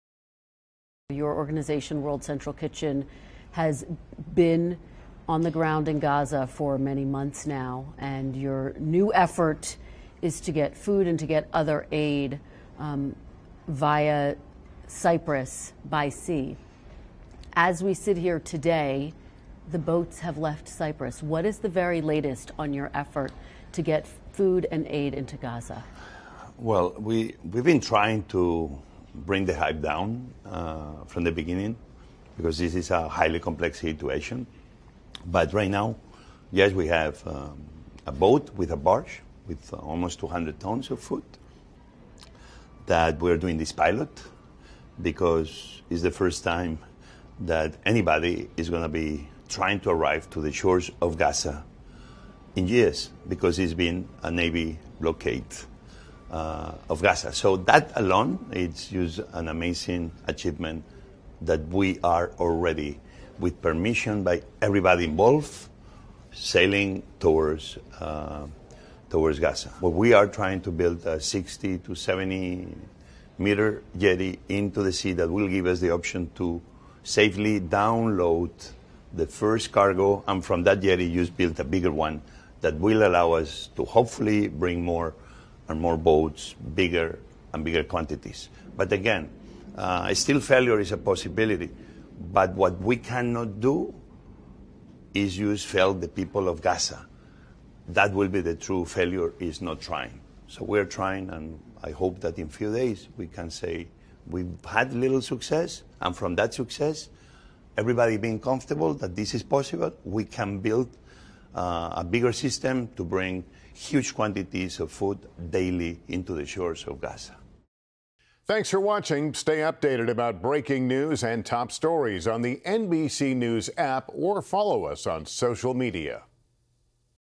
Chef José Andrés: ‘An amazing achievement,’ as his charity group sends 200 tons of food to Gaza
Chef José Andrés tells Kristen Welker that a relief effort from World Central Kitchen has finally gotten underway, shipping 200 tons of food to Gaza after waiting in Cyprus.